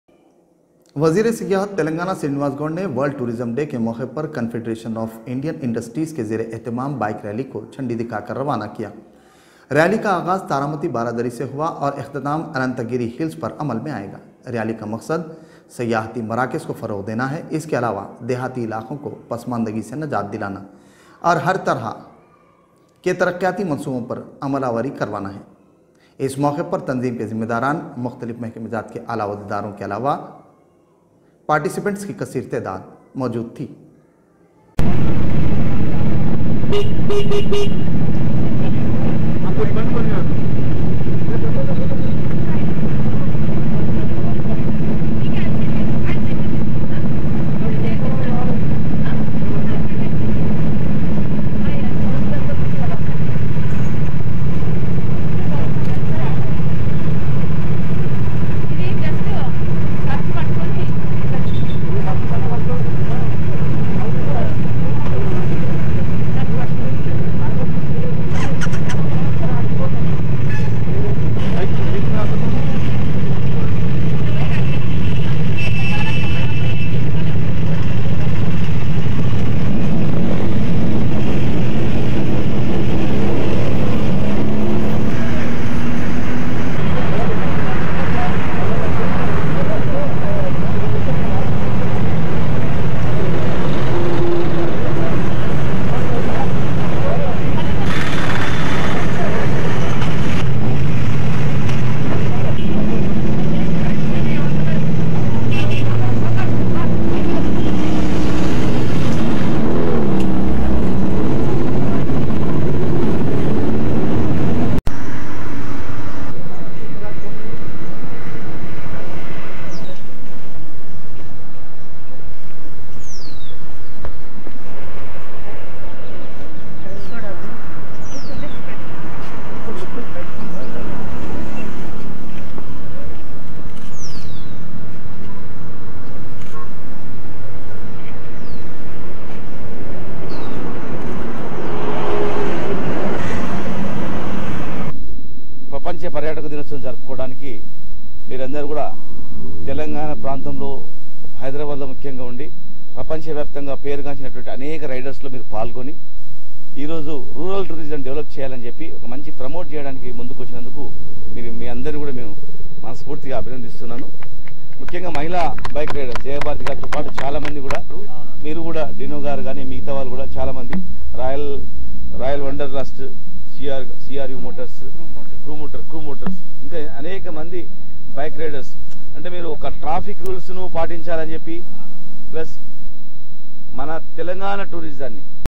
Telangana Tourism Minister Srinivasan Gowda flagged off the Bike Rally organized
Telangana Tourism Minister Srinivasan Gowda flagged off the Bike Rally organized by the Confederation of Indian Industries on the occasion of World Tourism Day. The aim is to promote tourist centers besides relieving rural areas from backwardness and implementing development projects at all levels.